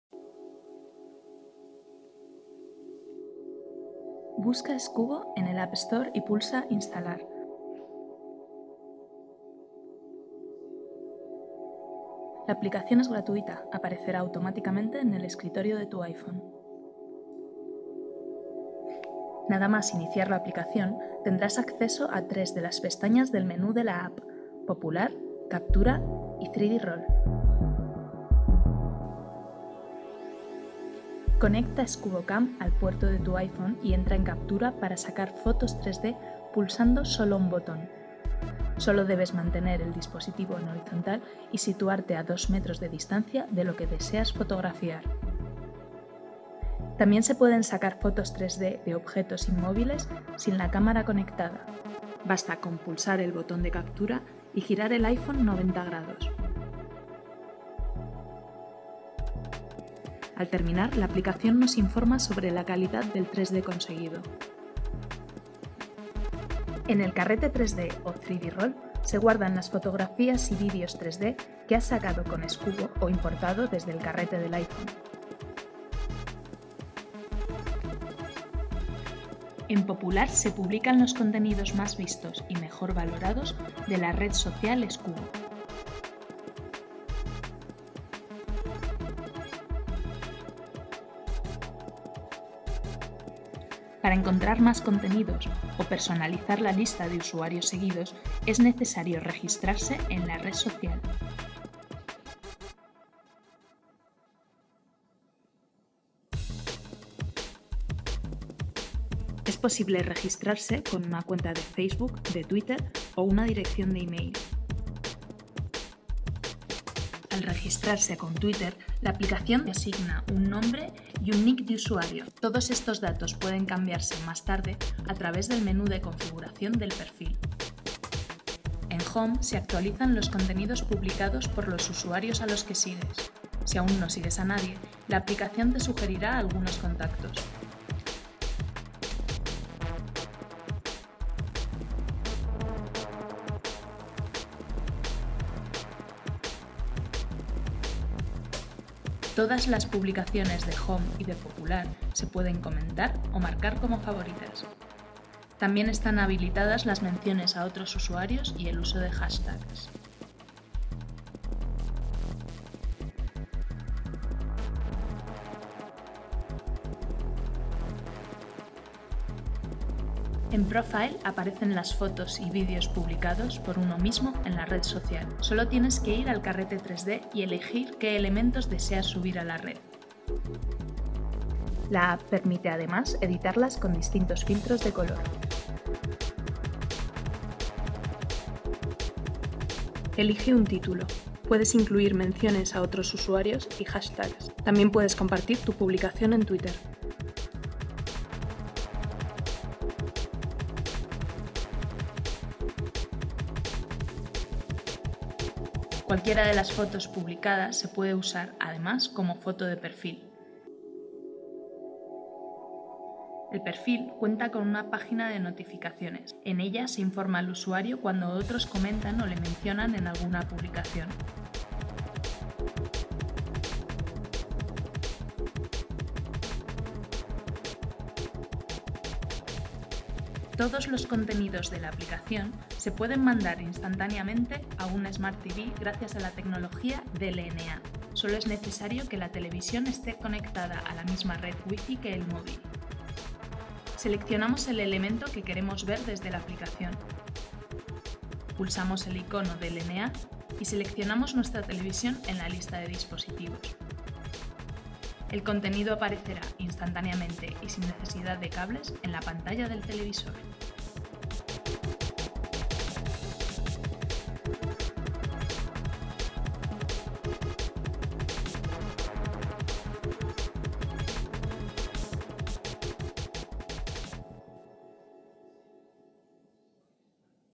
Tutorial Scubo App
Un vistazo rápido a toda la aplicación. Para conocer sus muchas posibilidades.